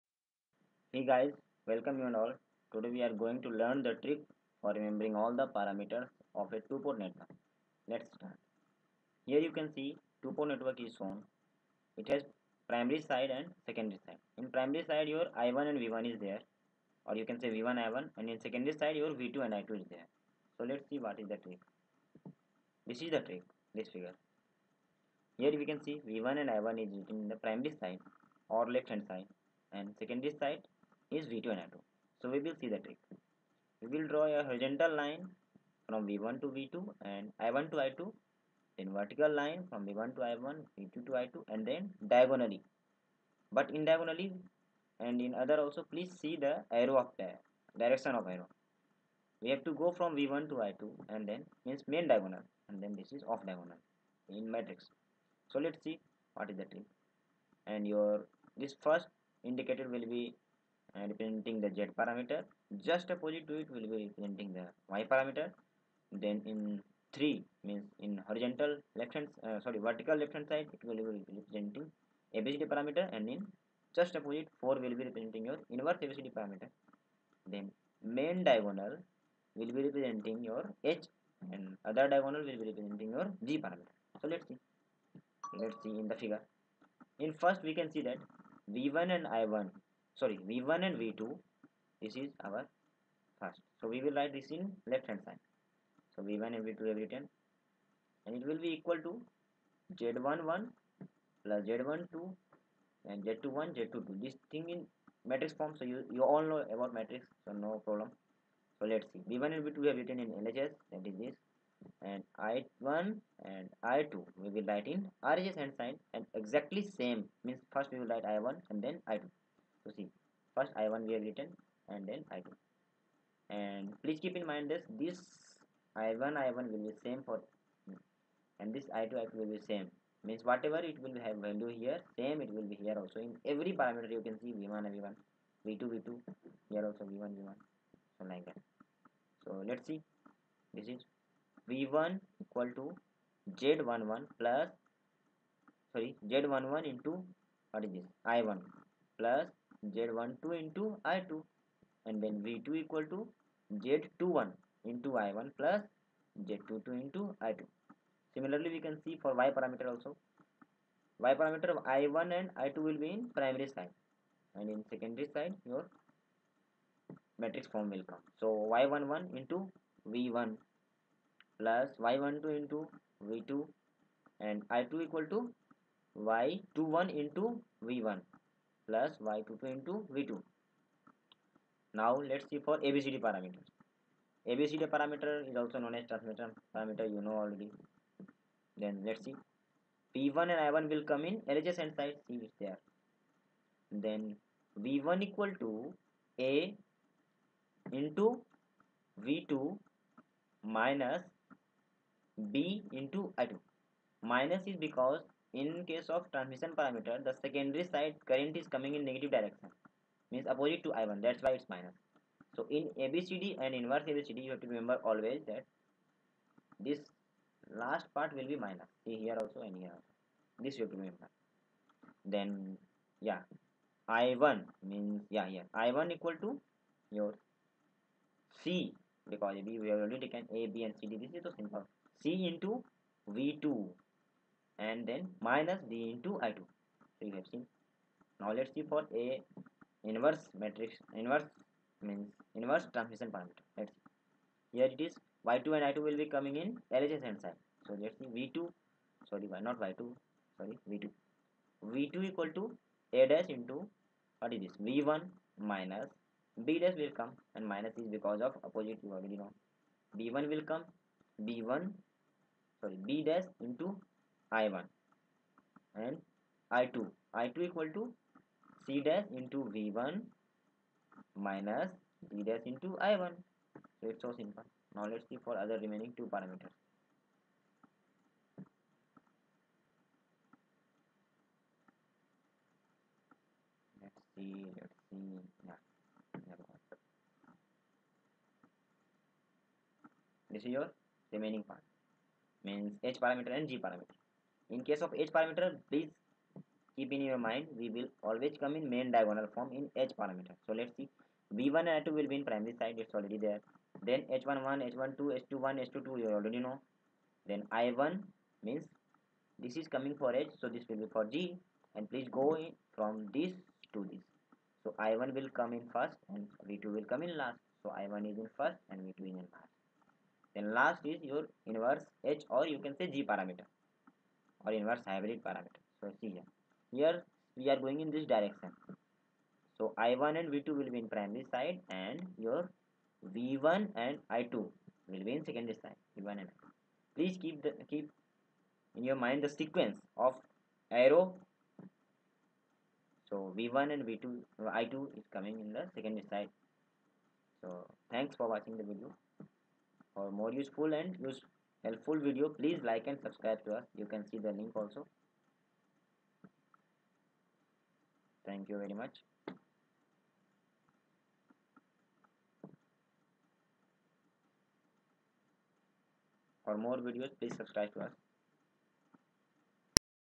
Shortcut tricks for Two (2) Port Network Parameter
This Video will be helpful in remembering all the parameters of a 2-port Network.